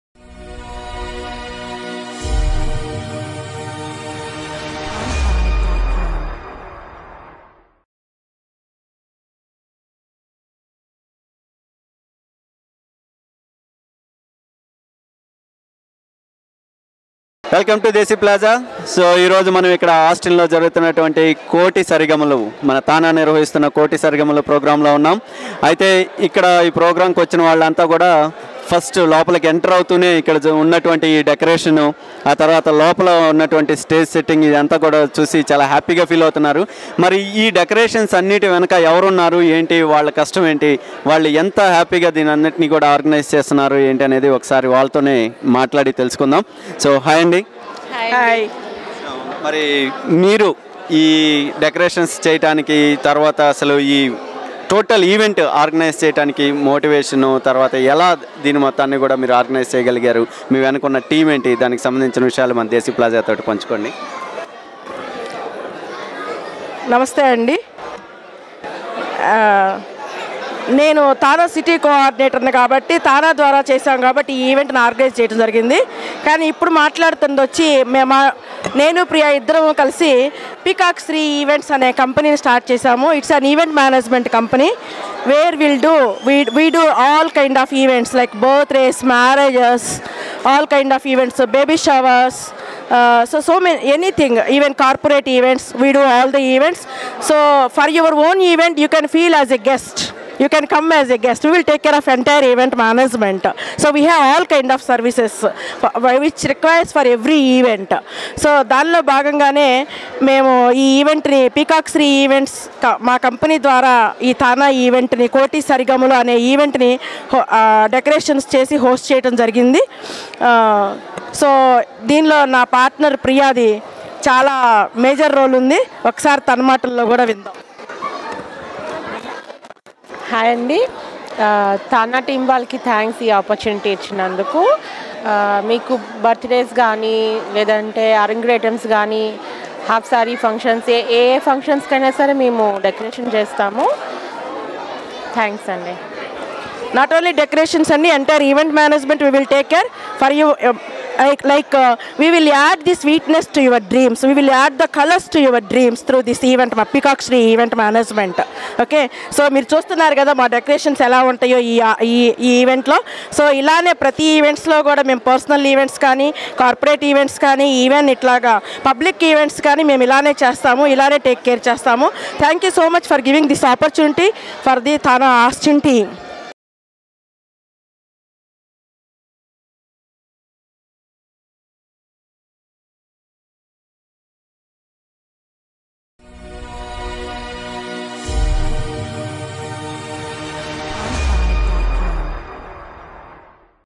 Kalpana and Priya talk about 'Koti Sarigamalu' event decoration and management || DesiplazaTV
Telugu Association of North America celebrated 'Koti Sarigamalu' event at Hutto Performing Arts Center, Hutto, TX on 10th Feb, 2018.

Koti along with his team Hema Chandra, Shravana Bhargavi, Pruthvi Chandra, Ranina Reddy enteratined the Texas audience. Around 1000 people attended the event and everyone made the event grand success.